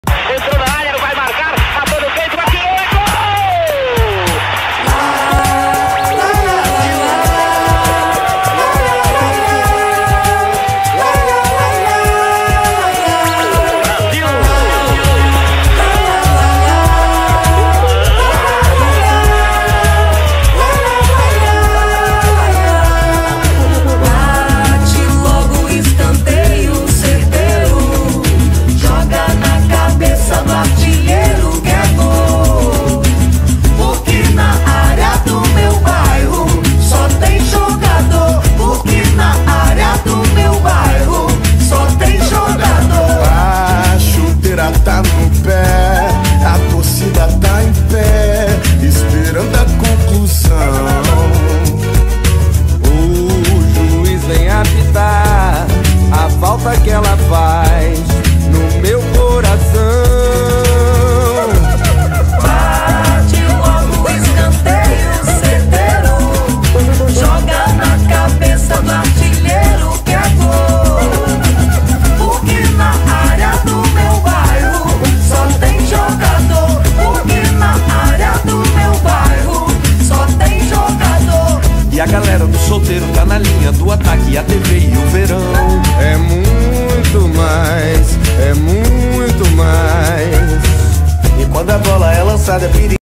Drawing ANTONY DOS SANTOS by fao00007 / ballpoint pen🎨
ANTONY🔱do santos #21

From brazil to @manchesterunited

That's one of my collection about football and i choosed the perfect one to show you i much i want improve day by day🤌🏾

Keep tag @antony00 in comment in order he sees it📩 .